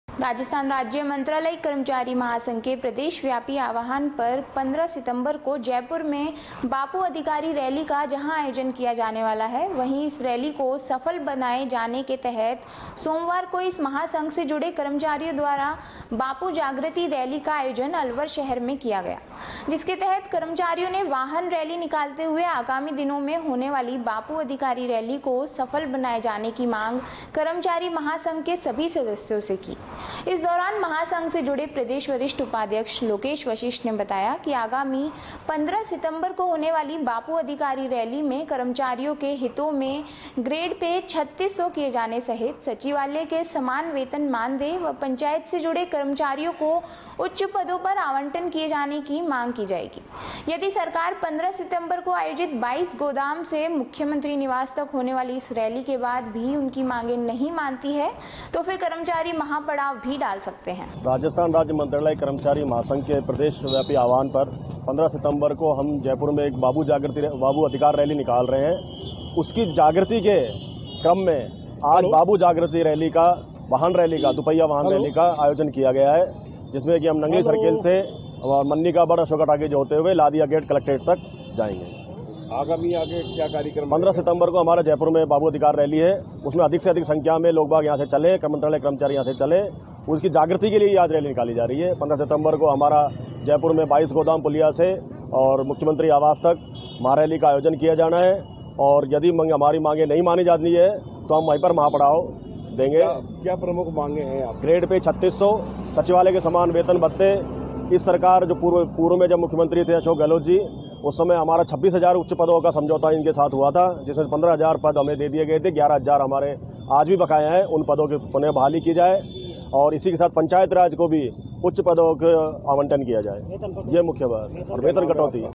alwarnwes//बाबू ! अधिकार रैली को लेकर तैयारिया शुरू// 13 09 2021
राजस्थान राज्य मंत्रालयिक कर्मचारी महासंघ के प्रदेश व्यापी आवाहन पर 15 सितंबर को जयपुर में बापू अधिकारी रैली का जहां आयोजन किया जाने वाला है ।
वहीं इस रैली को सफल बनाए जाने के तहत सोमवार को इस महासंघ से जुड़े कर्मचारियों द्वारा बापू जागृति रैली का आयोजन अलवर शहर में किया गया ।
जिसके तहत कर्मचारियों ने वाहन रैली निकालते हुए आगामी दिनों में होने वाली बापू अधिकारी रैली को सफल बनाए जाने की मांग कर्मचारी महासंघ के सभी सदस्यों से की ।
इस दौरान महा संघ से जुड़े प्रदेश वरिष्ठ उपाध्यक्ष लोकेश वशिष्ट ने बताया कि आगामी 15 सितंबर को होने वाली बापू अधिकारी रैली में कर्मचारियों के हितों में, ग्रेड पे 36सौ किए जाने सहित सचिवालय के समान वेतन मानदेय व पंचायत से जुड़े कर्मचारियों को उच्च पदों पर आवंटन किए जाने की मांग जाएगी । यदि सरकार 15 सितंबर को आयोजित 22 गोदाम से मुख्यमंत्री निवास तक होने वाली इस रैली के बाद भी उनकीं मांगे नही मानती है तो फिर कर्मचारी महापड़ाव भी डाल सकते है ।